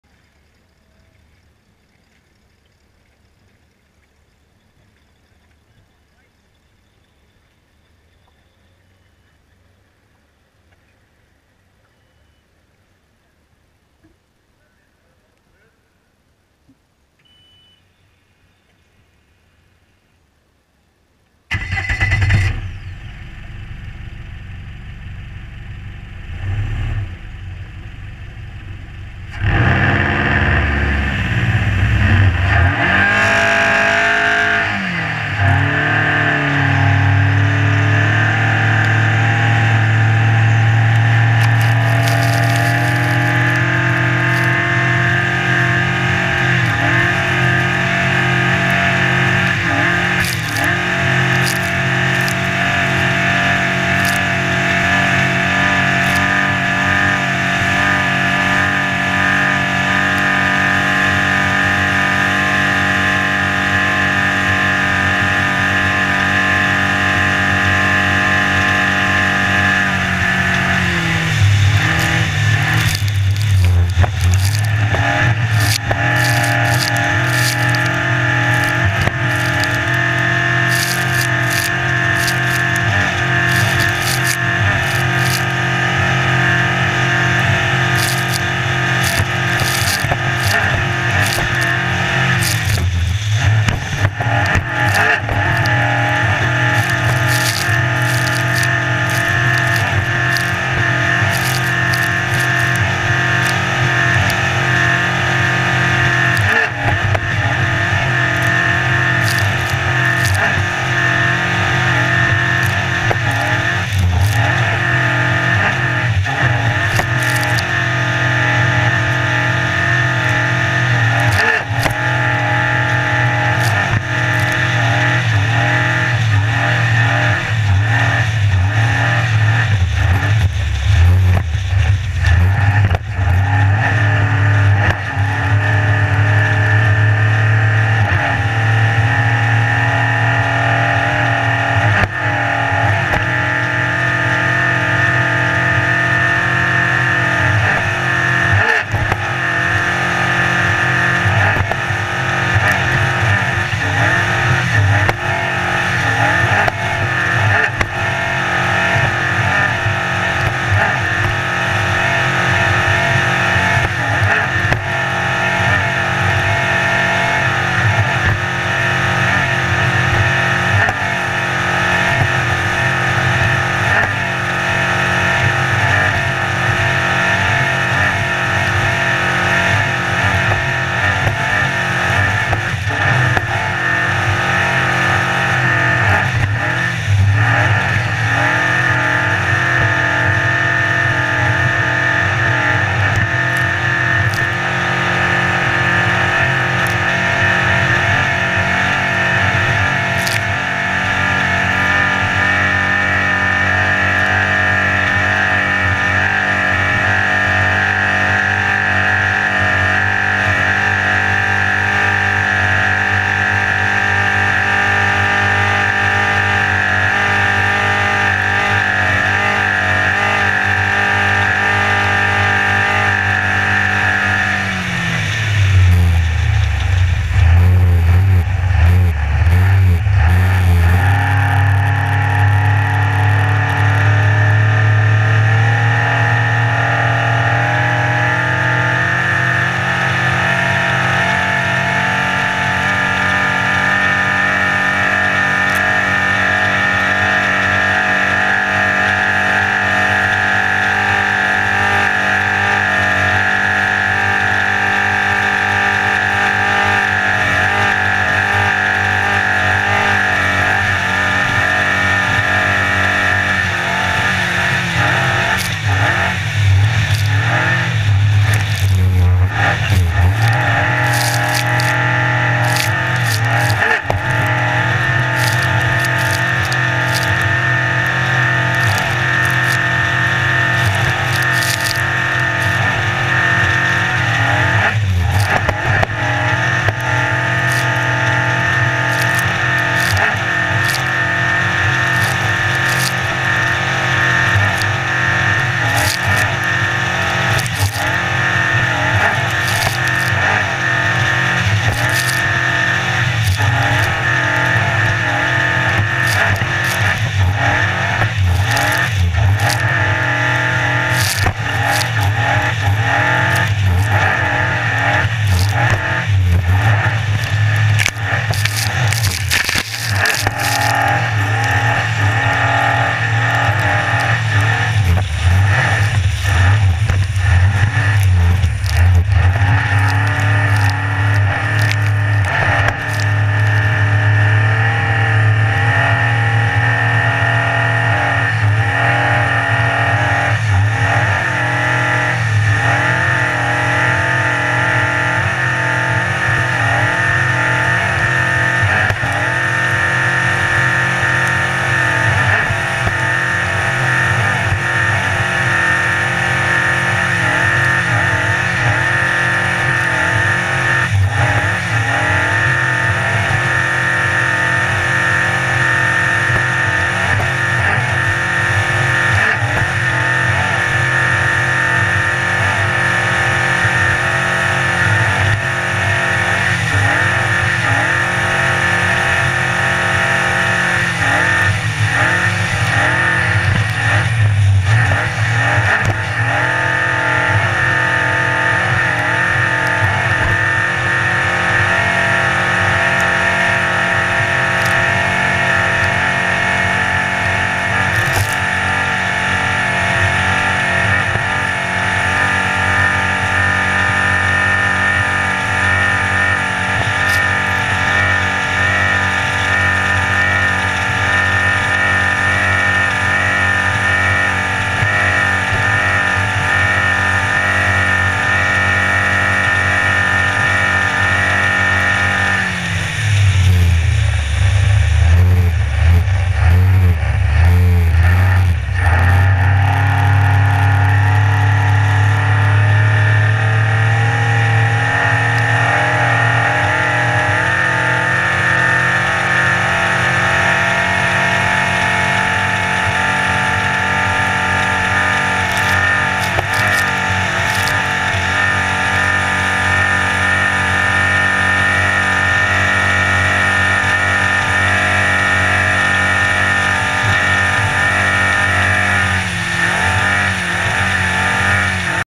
"Møkkalasset rundt 2018" del 1
Flosta Motorbåtforening arrangerte "Møkkalasset rundt 2018"

Film fra Ombord i Tintopieta`n til Christoffer Vibeto.
Talina Hagane er Co-pilot..